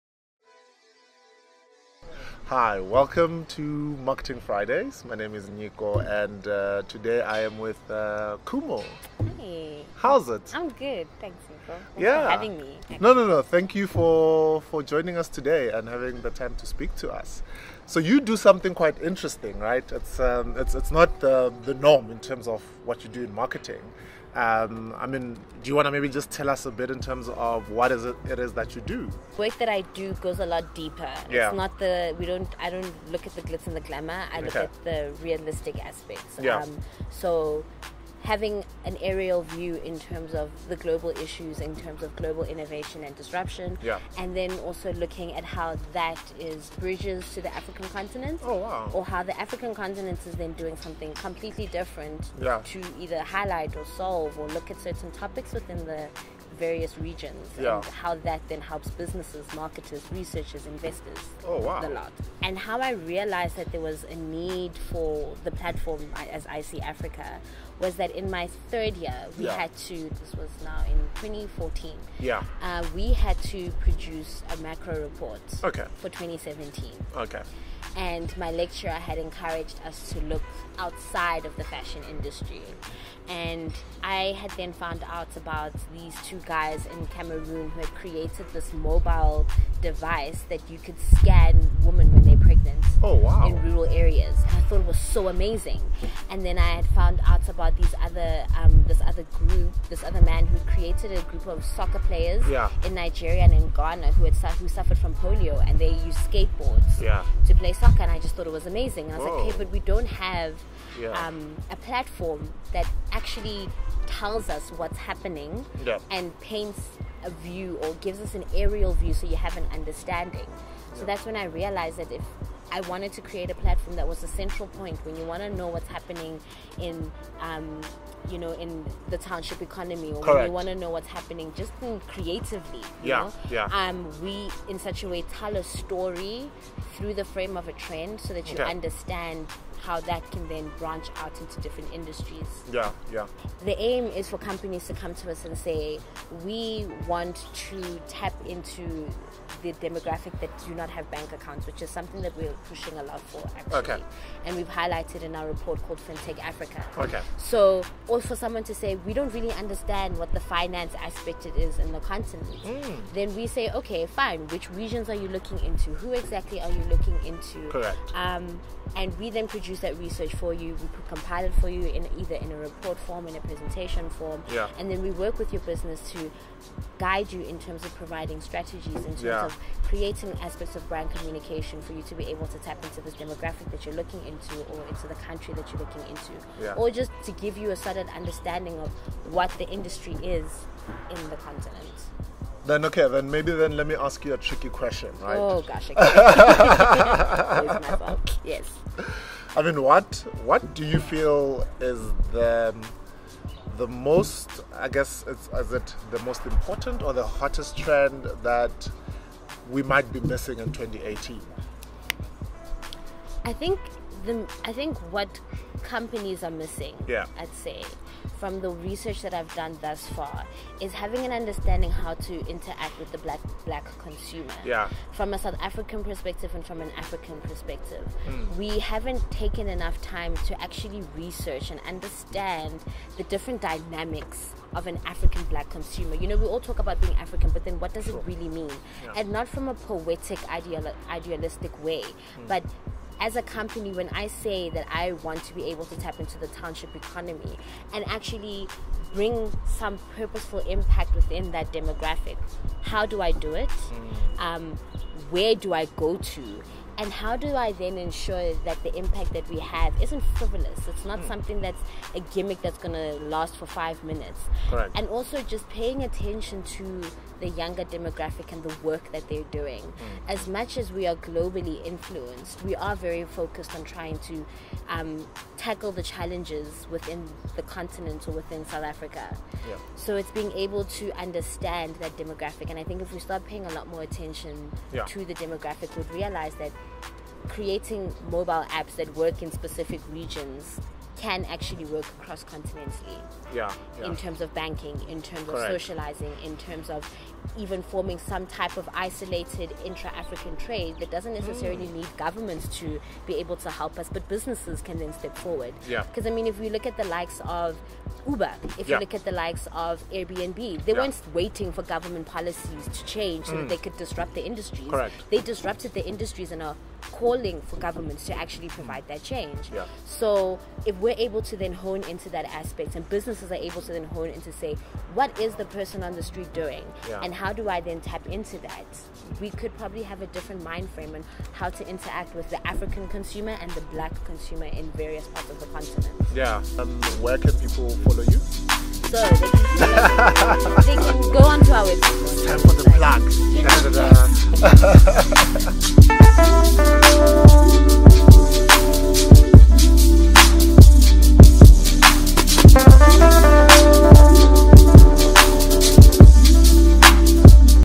Khumo Theko | Understanding the African Consumer
In this video, Nyiko Chauke of Marketing Fridays interviews Khumo Theko of I See Africa about her role as a trends specialist and some insights into the African consumer...

Theko says she takes an ariel view of global issues, innovation and disruption and then looks at how this bridges to Africa, noting that the continent often does things differently to address these and other local issues.

Follow Isee Africa on the following platforms